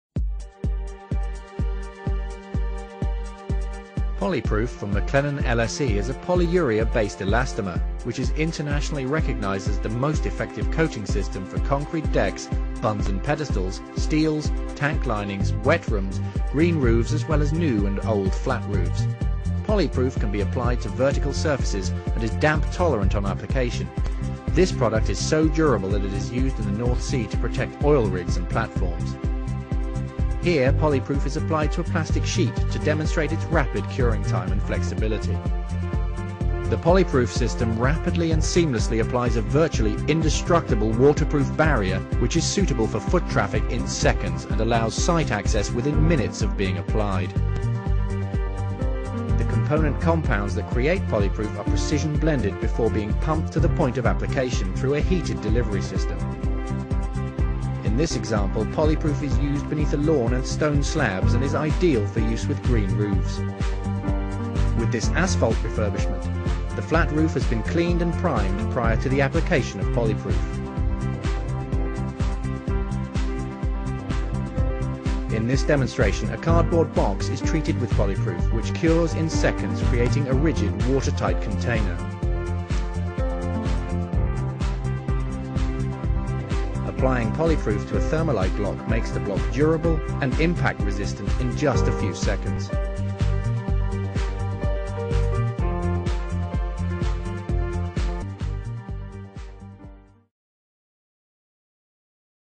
POLYPRUFE by Maclannan-LSE
Polyurea is being recognised internationally as the ultimate coating system for roofs, basements, bunds , steels, tank linings and a lot more.

As a testimony to it's durability and suitability for all of the above. It is used on the hulls of ships and barges, Aircraft carrier decks. North sea oil rigs/ platforms for corrosion protection and asbestos encapsulation and in some of the harshest conditions in the world for pipe work protection.

The system has been developed over the last 20 years for use in waterproofing for basements, reservoirs, roof and deck finishes, concrete and steel protection, tank lining, asbestos encapsulation, pipe protection, bund lining.

Polyprufe combines fast curing, even at very low temperatures, and water insensitivity with exceptional mechanical properties, chemical resistance and durability.